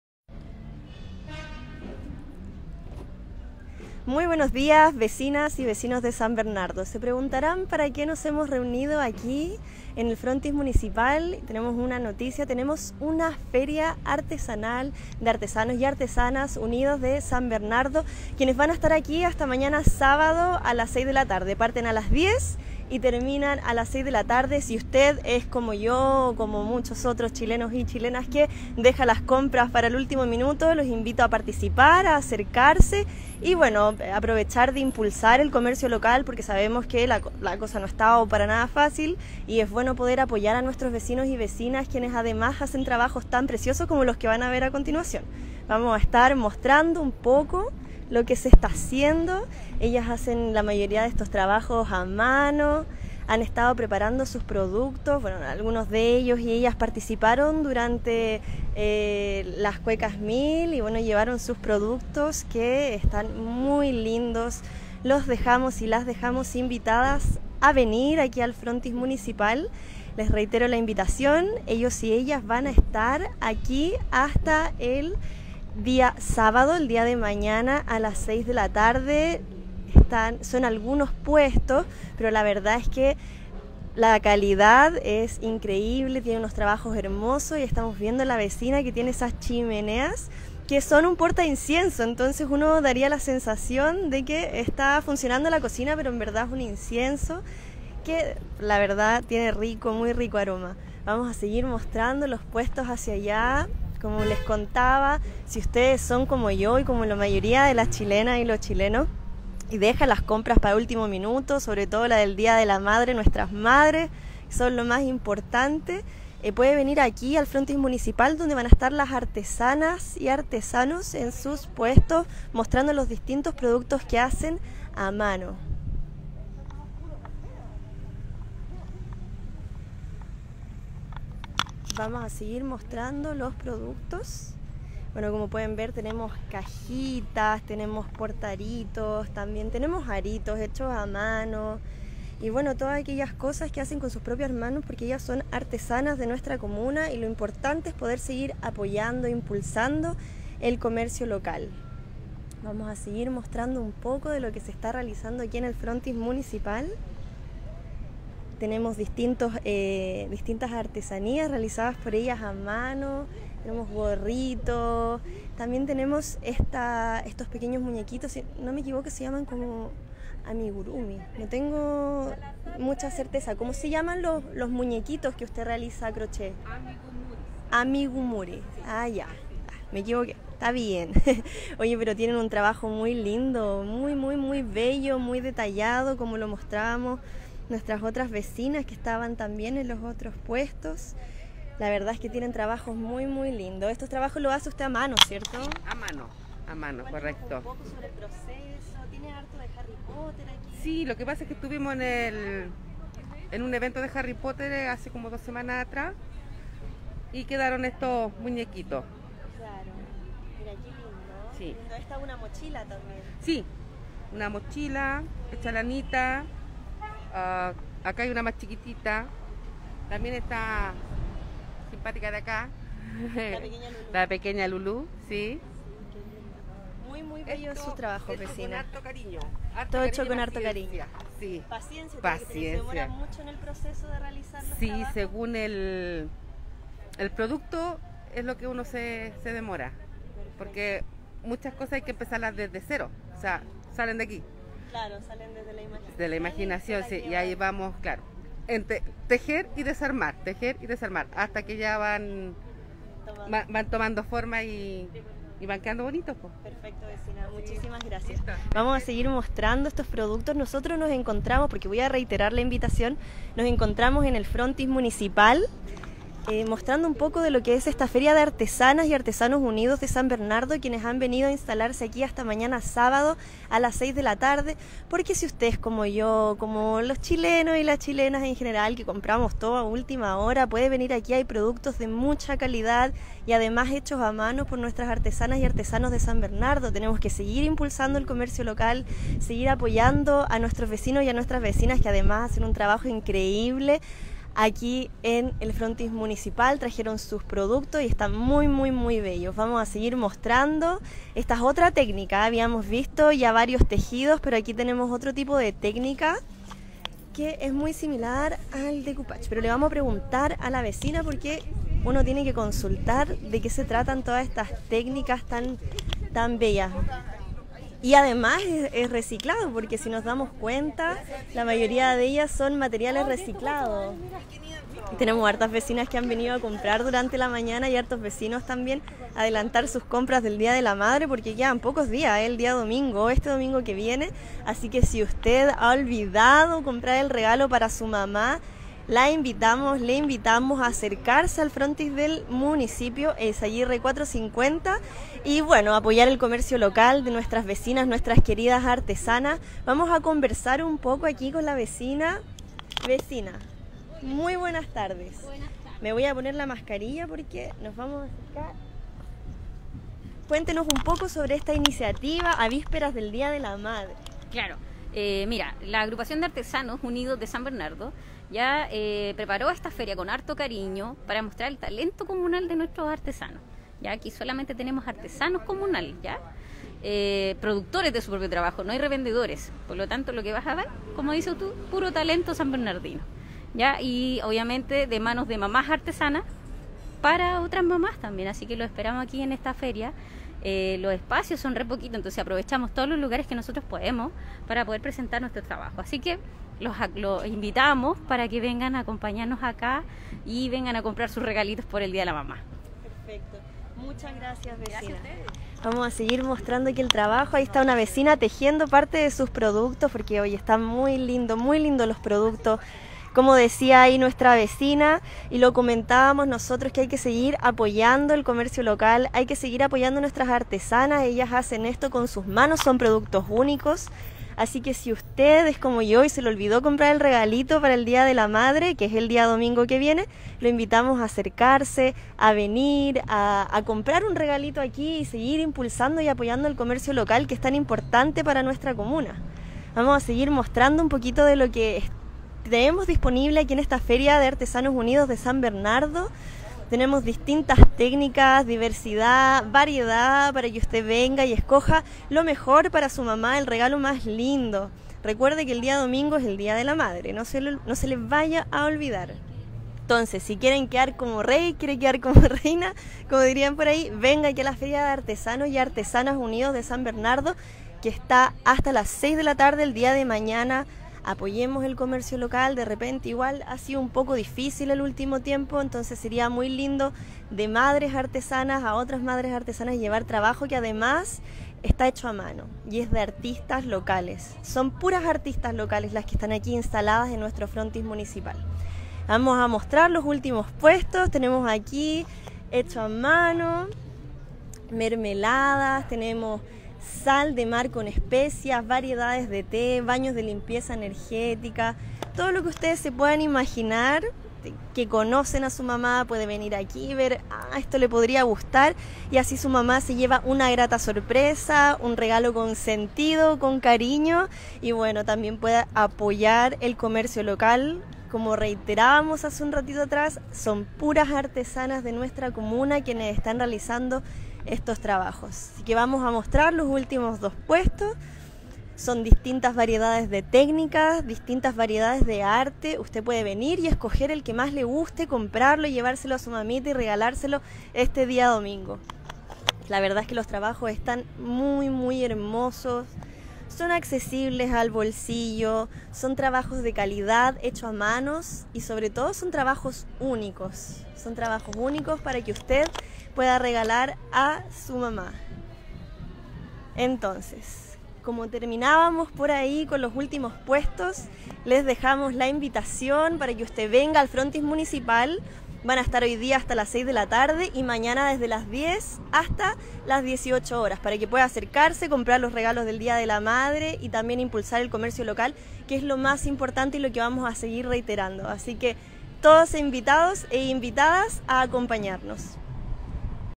Feria de artesanas y artesanos unidos de San Bernardo en el frontis municipal
Hasta mañana sábado desde las 10 am hasta las 18 hrs.